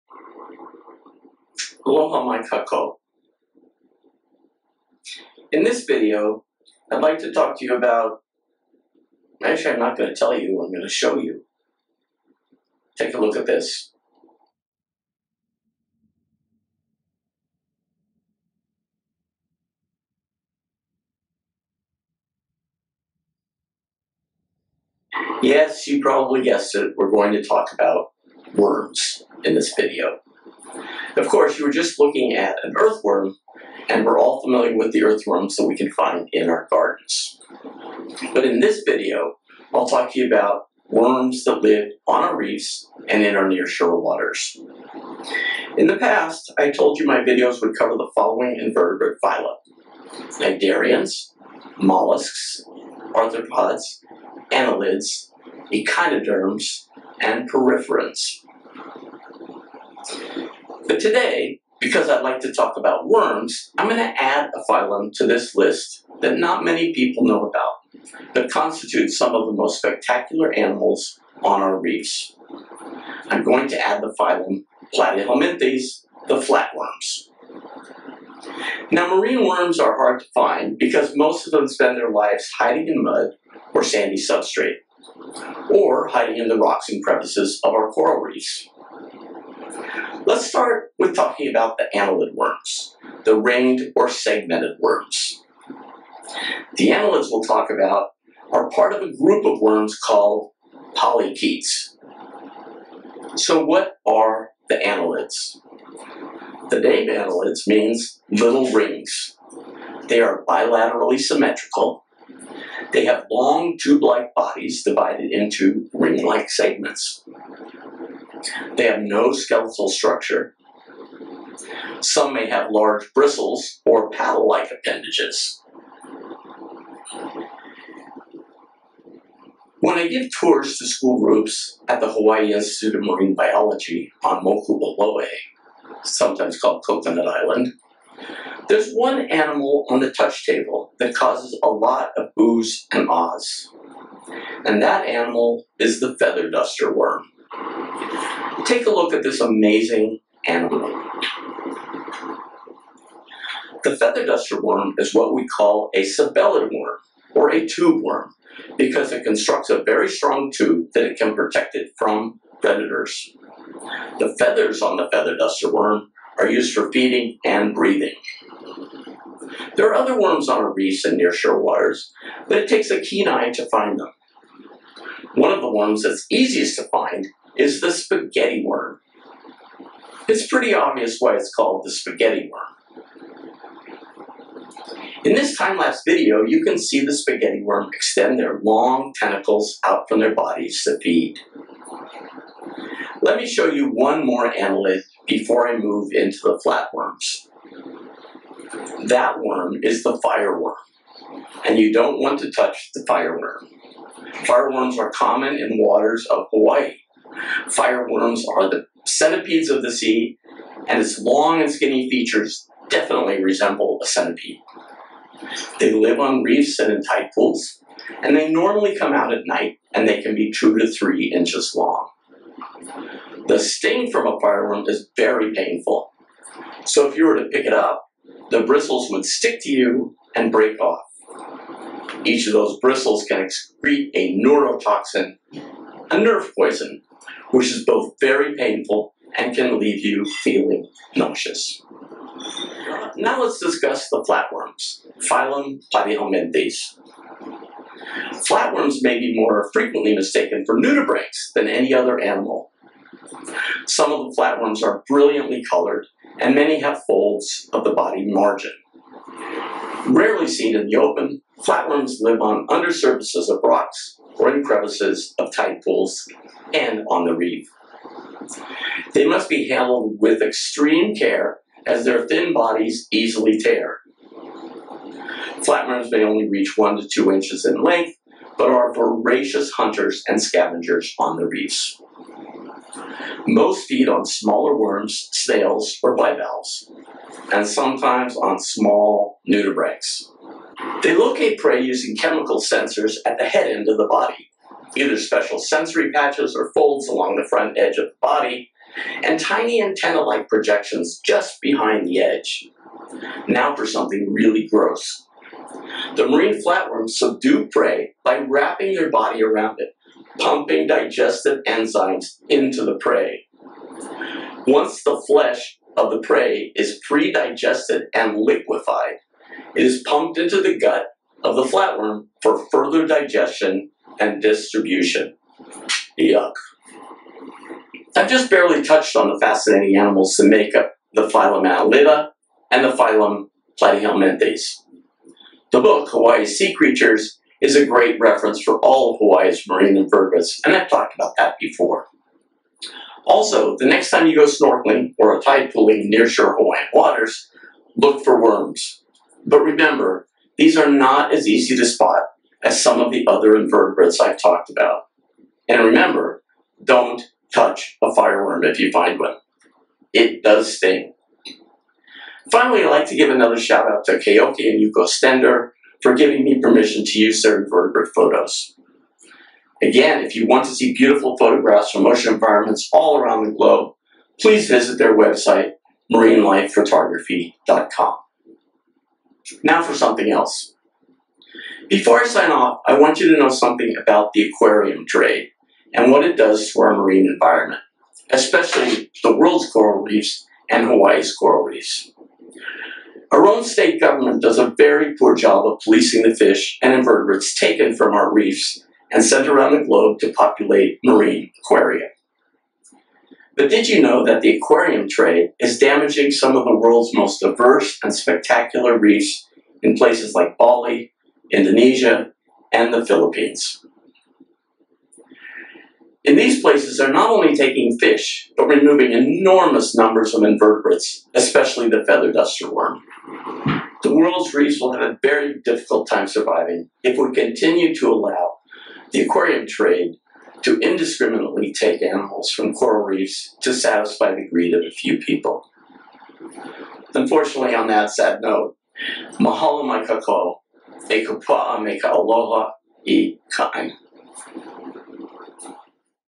annelids and flatworms HD 720p
This video is an introduction to two phyla: Annelida and Platyhelminthes.  At the end of the video I do a short segment on the damage to coral reef ecosystems caused by the worldwide aquarium trade.